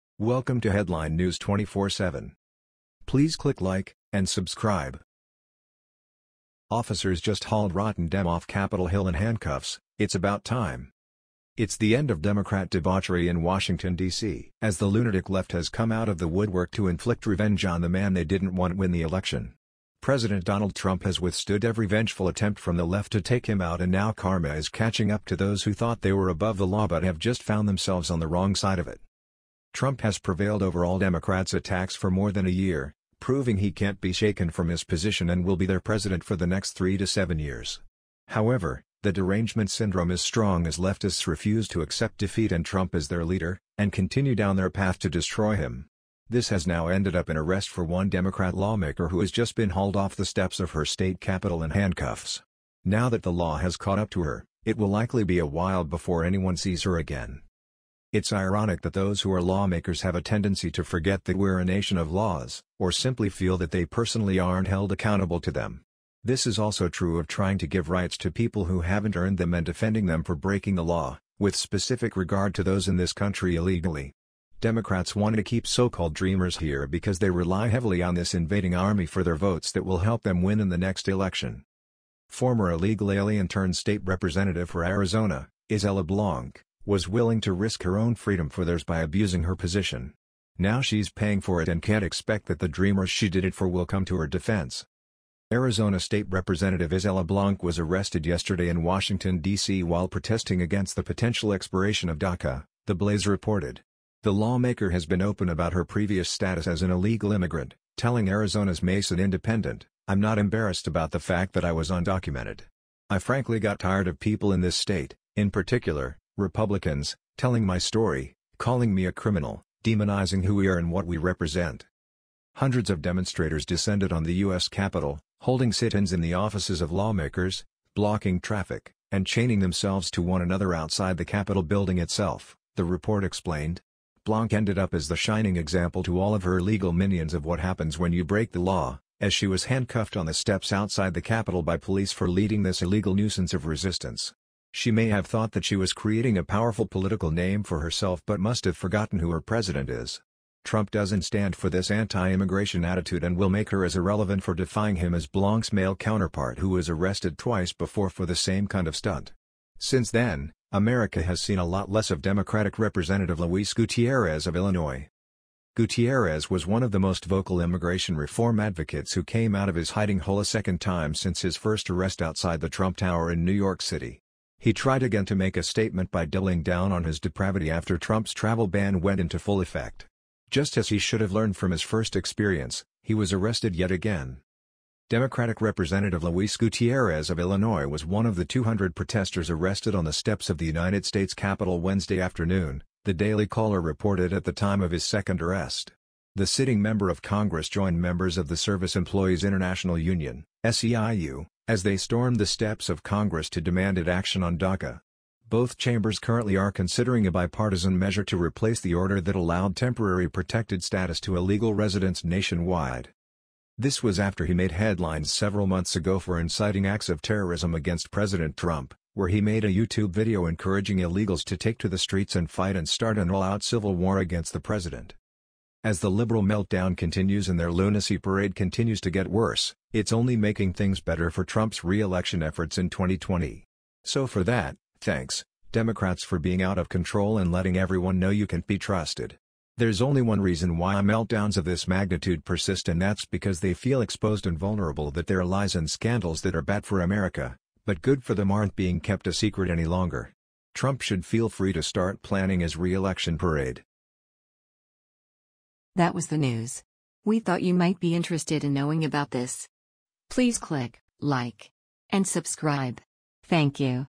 Officers Just Hauled Rotten Dem Off Capitol Hill In Handcuffs – It’s About Time!
Hundreds of demonstrators descended on the US capital, holding sit-ins in the offices of lawmakers, blocking traffic, and chaining themselves to one another outside the Capitol building itself